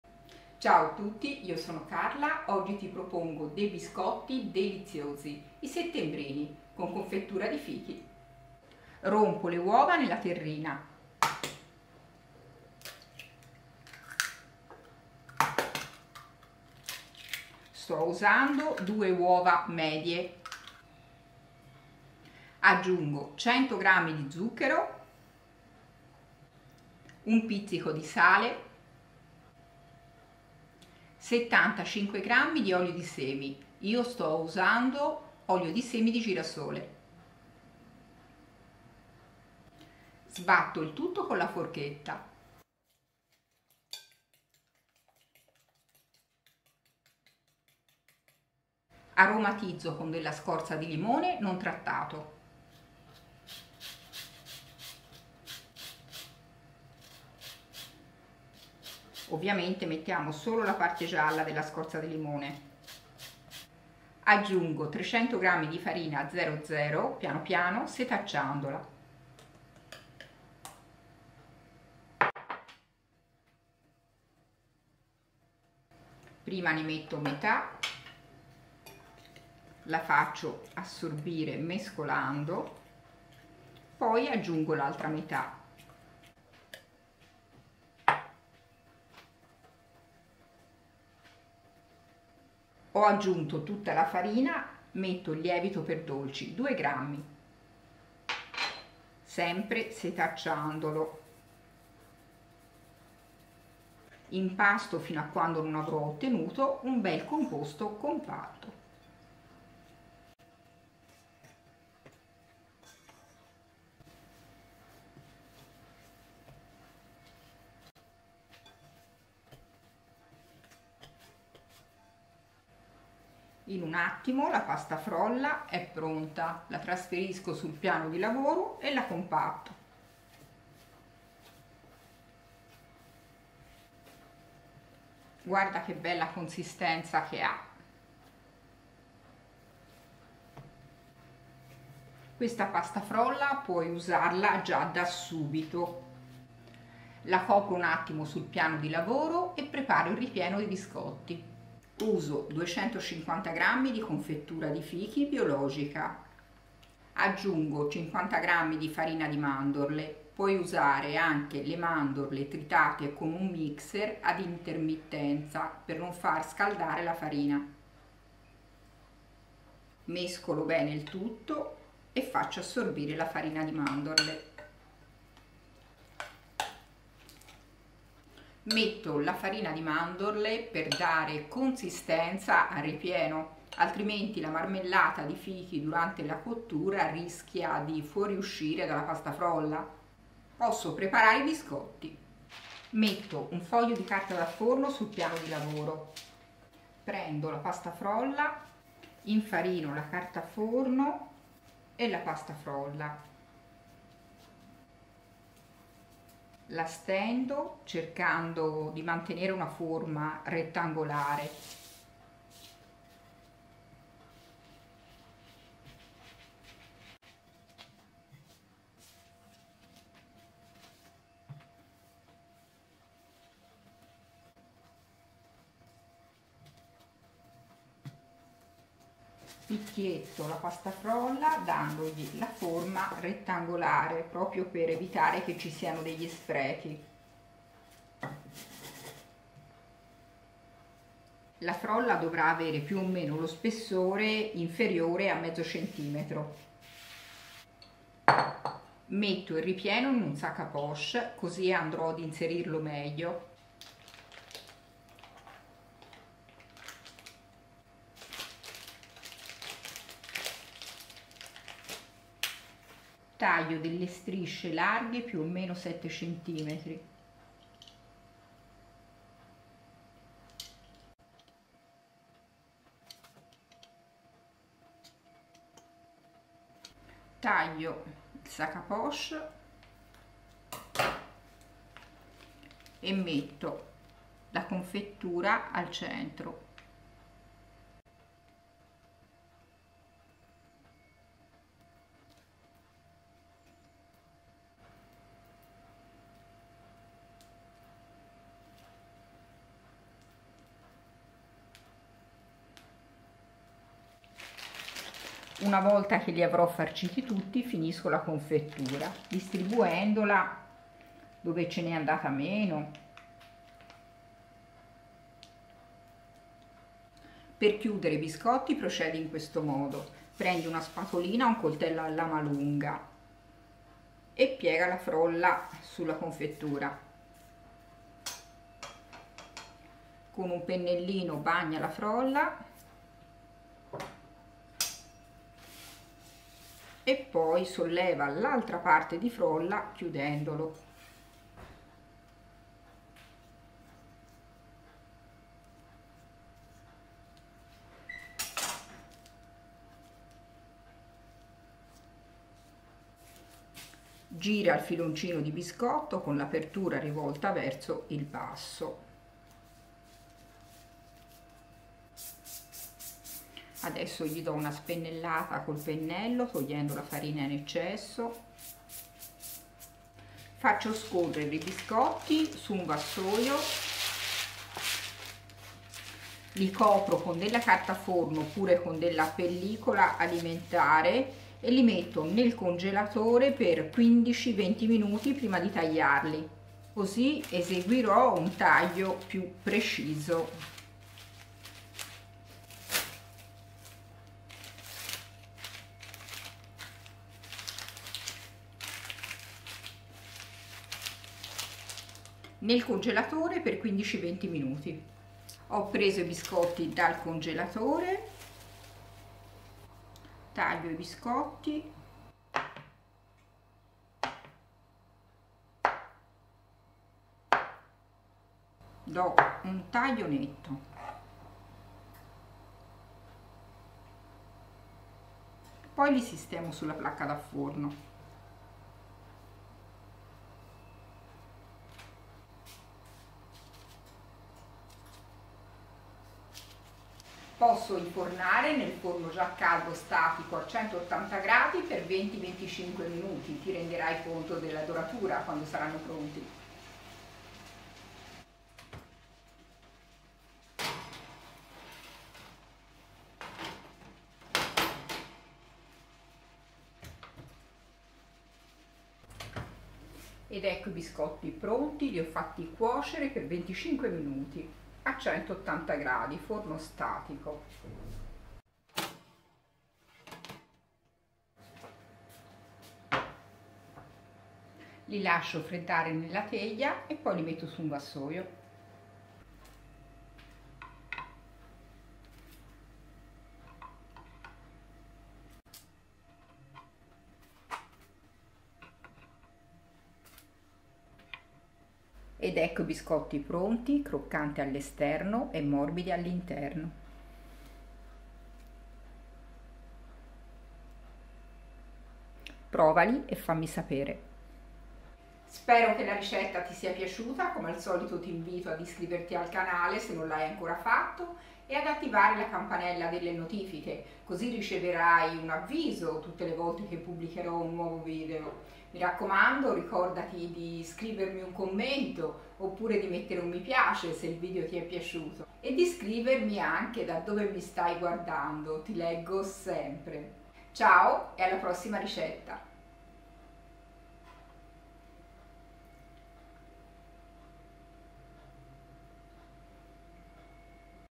Biscotti Settembrini con confettura di fichi
Ciao a tutti, oggi ho preparato i biscotti con confettura di fichi: i Settembrini.
Ho usato la pasta frolla all'olio pronta in un attimo, poi preparato il ripieno dei biscotti con confettura di fichi biologica e farina di mandorle.
Vieni con me in cucina e prepariamoli insieme!

Ingredienti per circa 48/50 biscottini
Pasta frolla:
300g di farina 00
100g di zucchero
75g di olio di semi
2 uova medie
un pizzico di sale
2/3g di lievito per dolci
scorza di limone non trattato
Ingredienti per il ripieno:
250g di confettura di fichi (io ho usato quella biologica)
50g di farina di mandorle